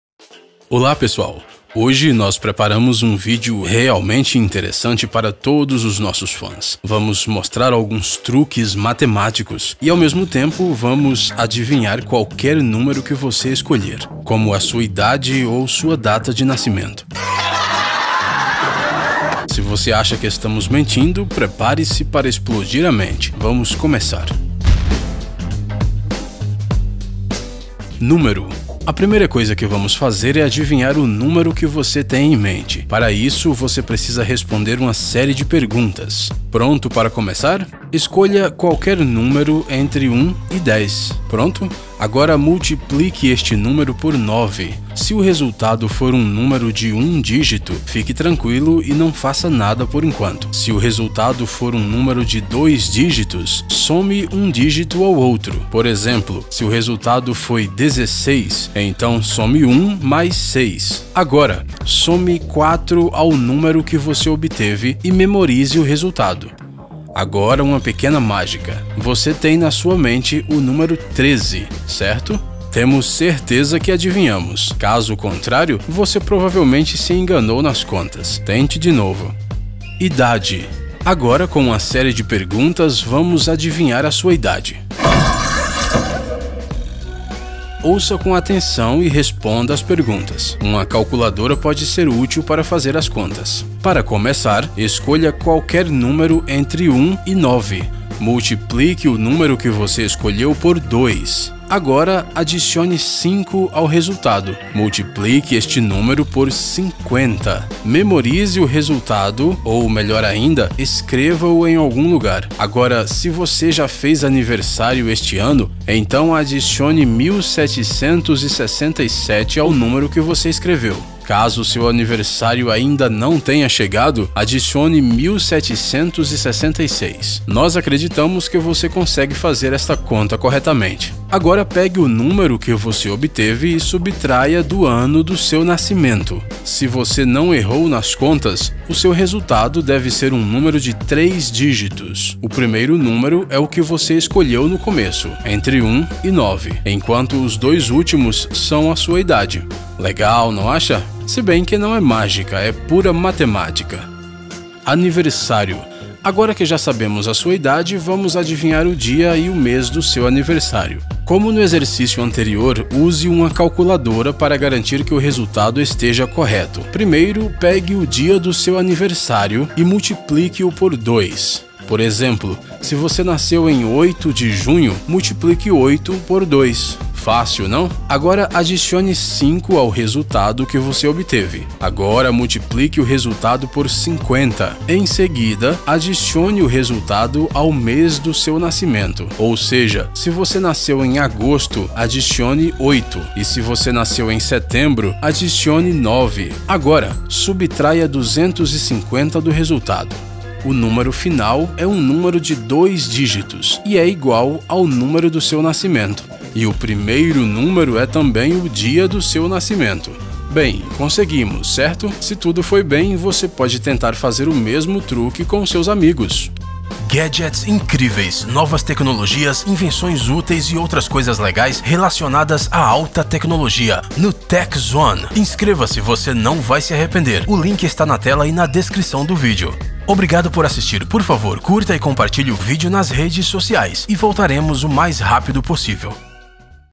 Eu Vou Adivinhar a Sua Idade em um Minuto
TechZone ►

Hoje nós preparamos um vídeo realmente interessante para todos os nossos fãs. Nós vamos mostrar alguns truques matemáticos e ao mesmo tempo vamos adivinhar qualquer número que você escolher, bem como a sua idade e sua data de nascimento. Se você acha que estamos mentindo, prepare-se para explodir sua mente.